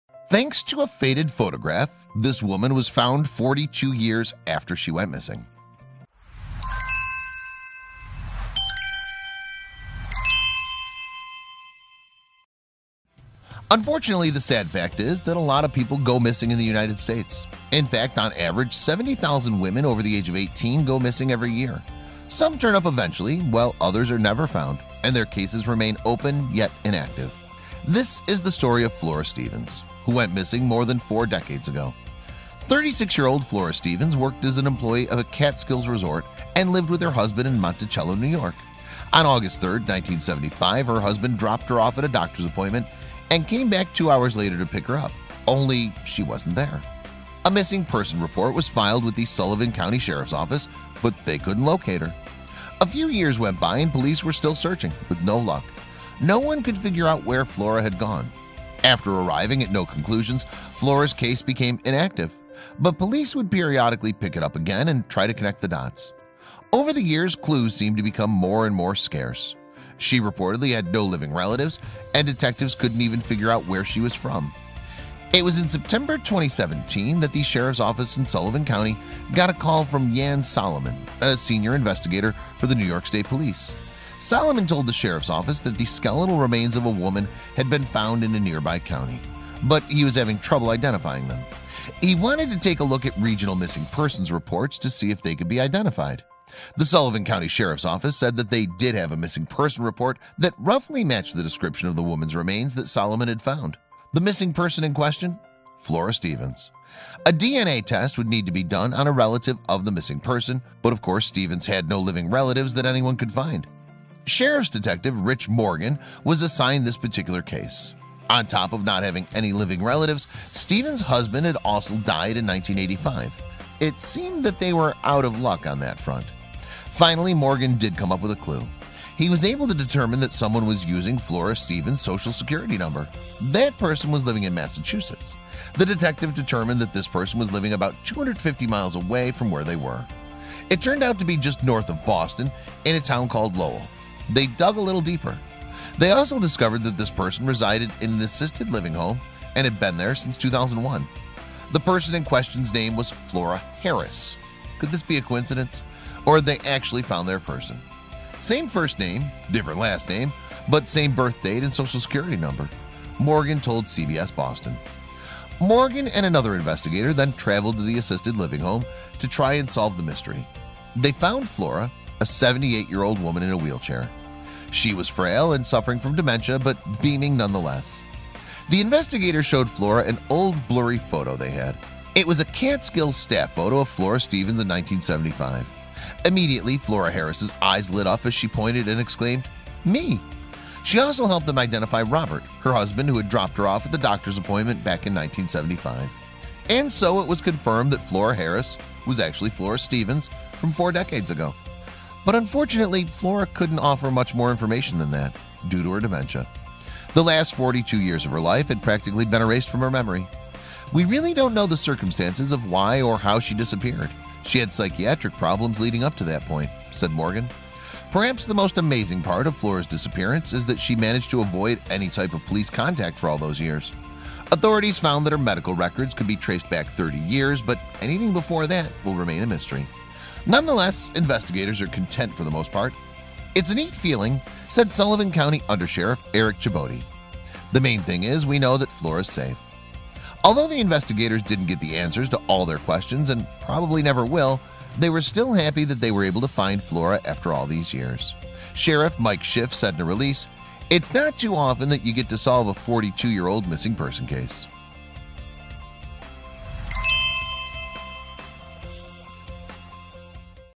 Thanks To A Faded Photograph, This Woman Was Found 42 Years After She Went Missing..
Unfortunately, the sad fact is that a lot of people go missing in the United States. In fact, on average 70,000 women over the age of 18 go missing every year.
Some turn up eventually, while others are never found and their cases remain open, yet inactive.
This is the story of Flora Stevens, who went missing more than four decades ago.